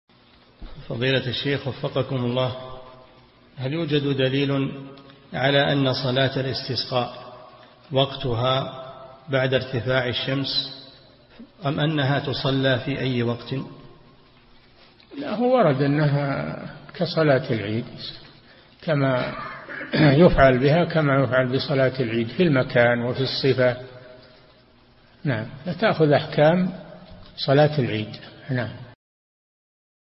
[506 -939] هل يوجد دليل على أن صلاة الاستسقاء وقتها بعد ارتفاع الشمس أم أنها تصلى في أي وقت؟
-[506 -939] السؤال: هل يوجد دليل على أن صلاة الاستسقاء وقتها بعد ارتفاع الشمس أم أنها تصلى في أي وقت ؟
الجواب: هو ورد أنه كصلاة العيد يفعل بها كما يفعل بصلاة العيد في المكان وفي الصفة  تأخذ أحكام صلاة العيد.
للاشتراك في قناة الشيخ صالح الفوزان

للاستفسار والإقتراحات والدعم
رئيس الفريق
لمشروع كبار العلماء